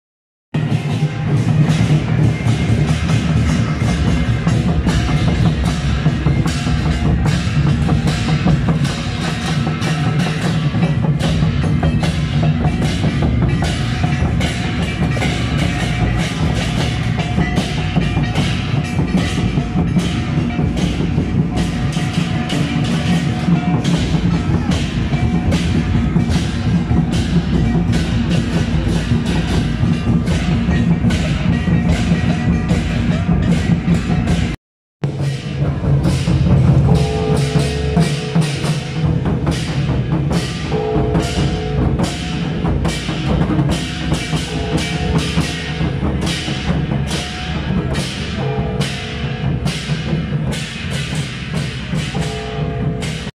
【基隆精武門-大鼓】~110汐止大尖山聖德宮天上聖母聖誕暨建宮50週年遶境
基隆精武門-大鼓(0:00)過境 汐止三合帝君會 (0:35)過境 汐止濟德宮